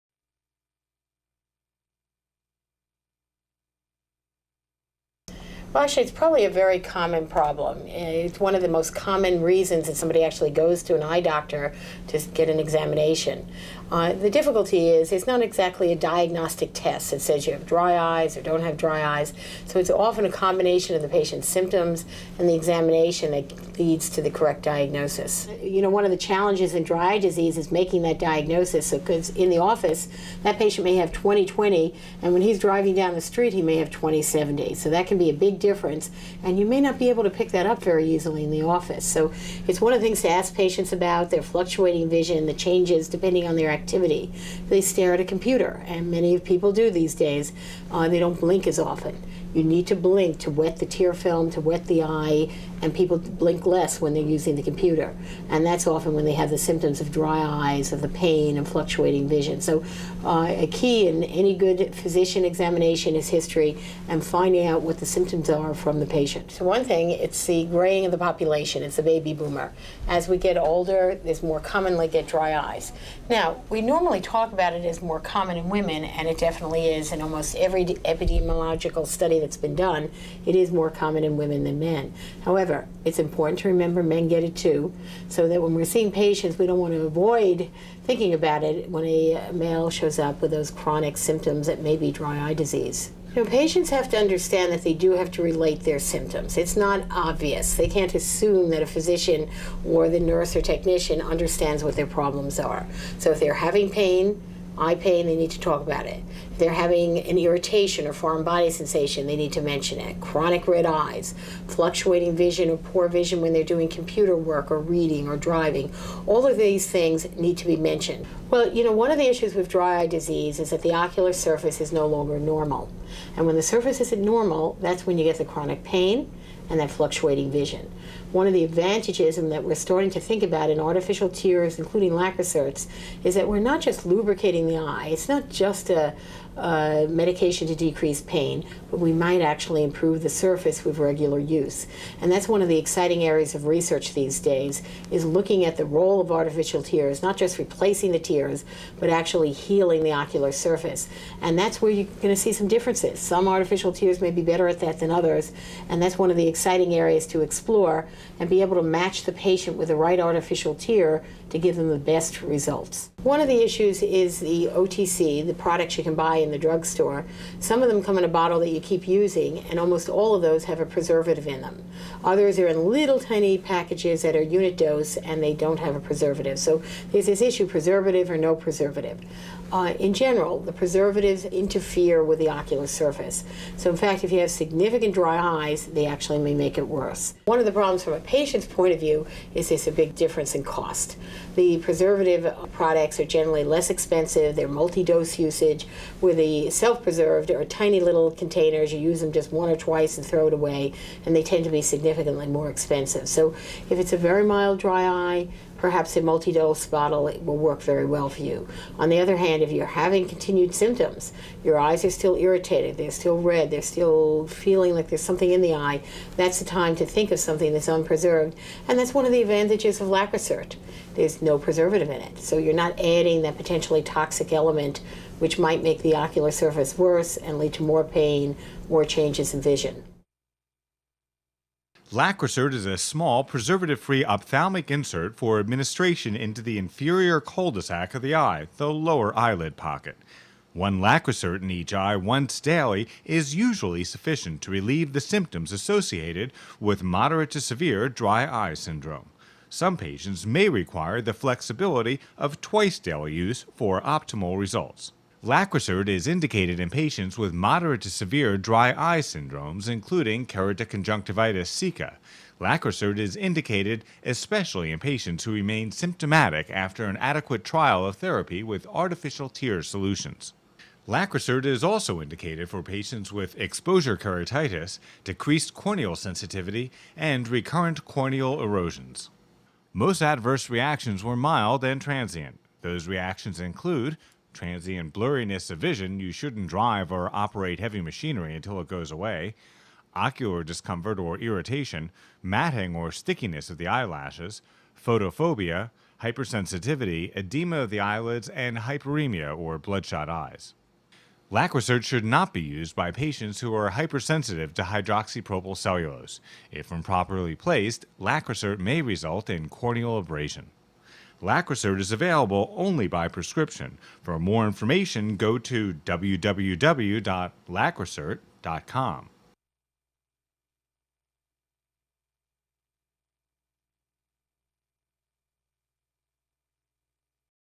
About Dry Eyes
Doctor Penny Asbell is one of the world leaders in the care of Dry Eye Disease.  Here, the Mount Sinai school of medicine professor talks about treatment options.